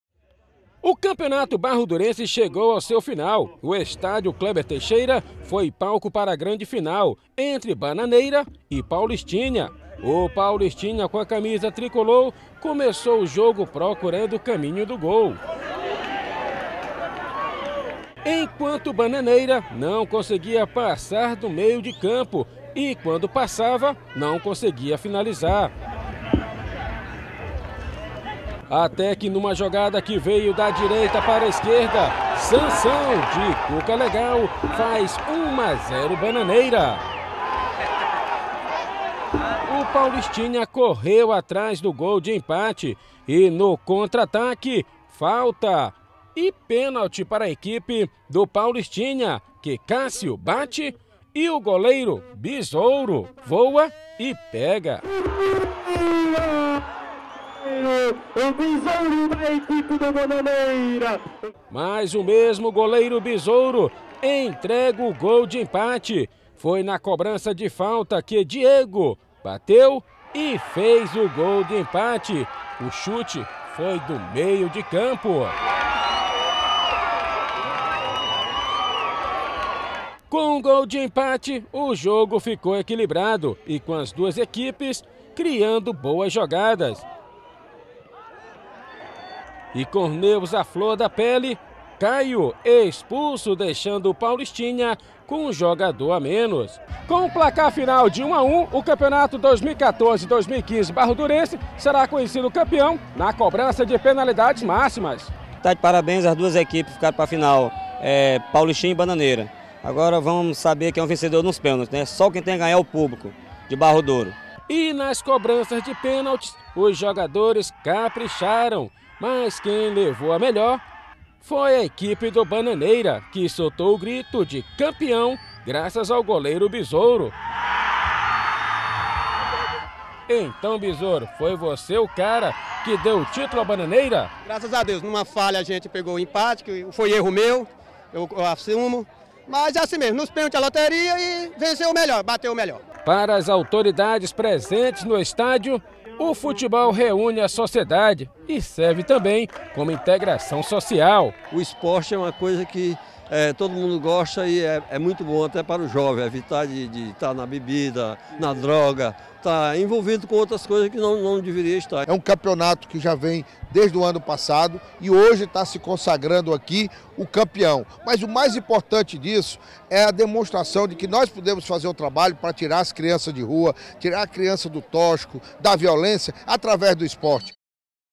Bananeira e Paulistinha realizam final do Campeonato Barro Durense
Bananeira e Paulistinha realizam final do Campeonato Barro Durense - TV Antena 10